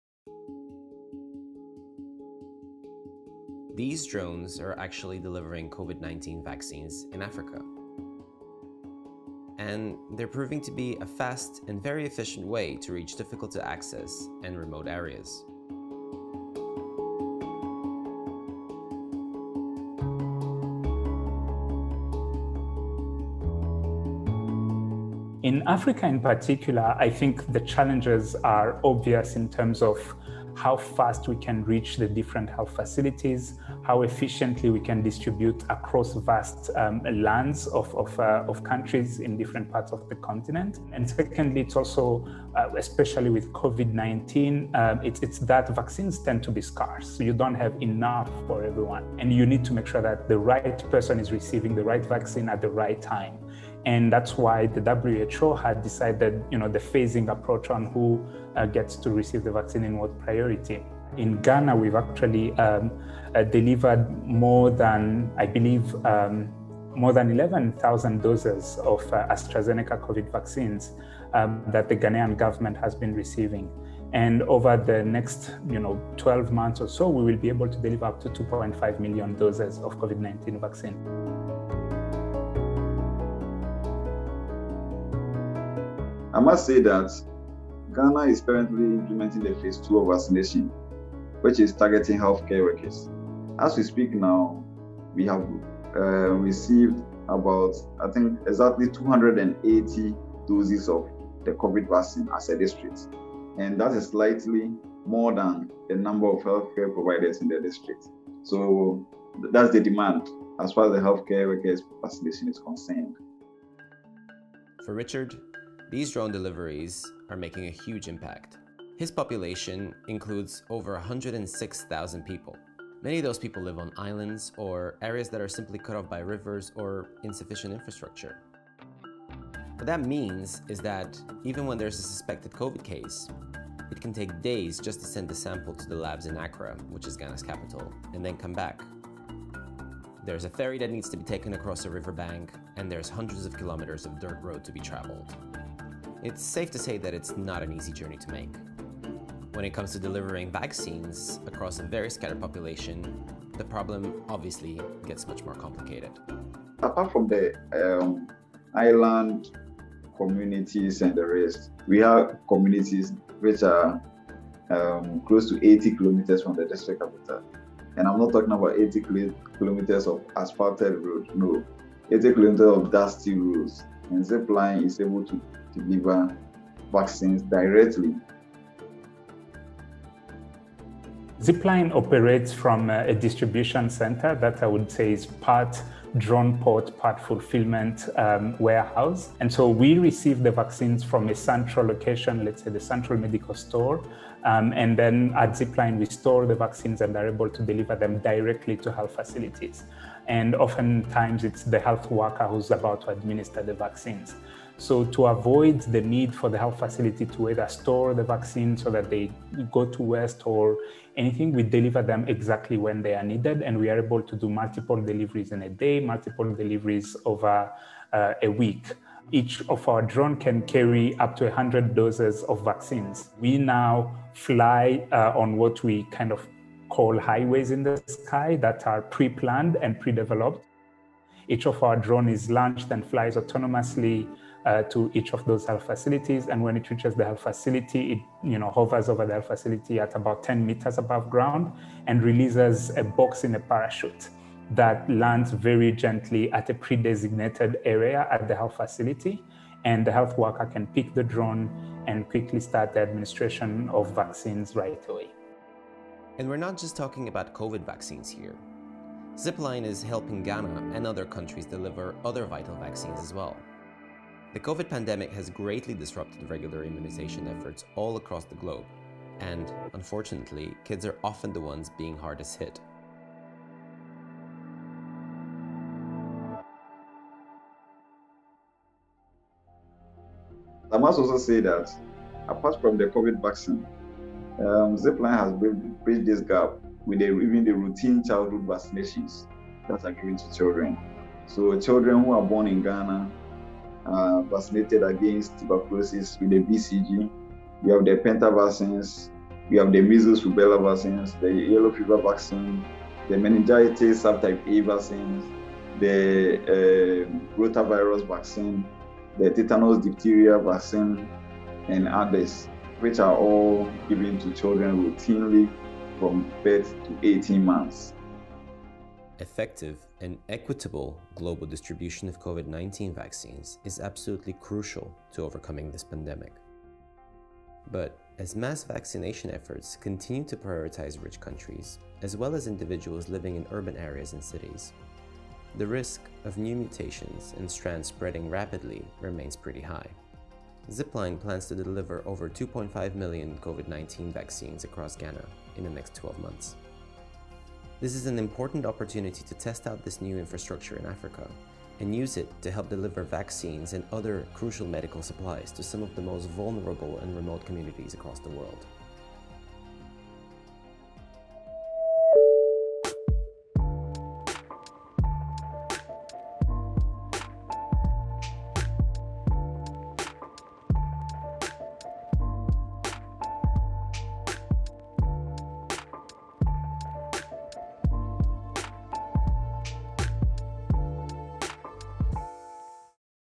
Drones Deliver COVID-19 Vaccines in Africa | Mashable
Zipline, a medical delivery drone system, is working to deliver millions of COVID-19 vaccines across Africa. To overcome this pandemic, new infrastructure must be utilized to ensure that the global distribution of vaccines is both effective and equitable.

Zipline's 'highways in the sky' approach could be an important tool in supplying other vital medical supplies to remote places, too. In Ghana, for example, the drones are used to help resume vital immunization programs for children.